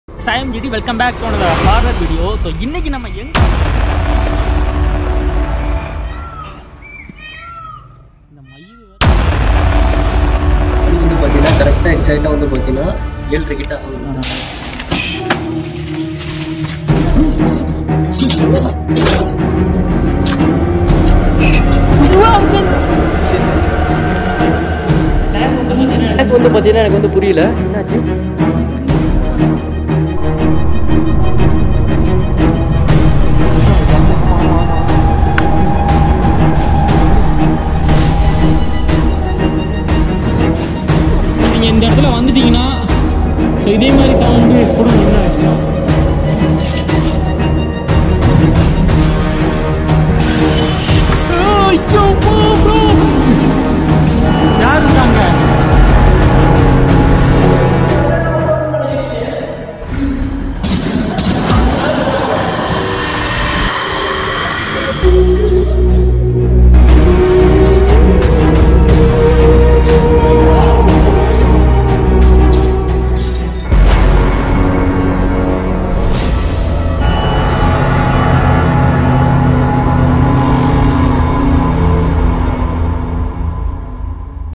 SPELS OF DEMON FACTORY|TRAILER
THIS VIDEO 📸 VERY DANGEROUS PLACE IM CAPTURED STRANG FOOTAGE CAMERA

Im not recommended to video kids&heart patients.

For social media insta: tn49guru

    Thank you 🙏 our support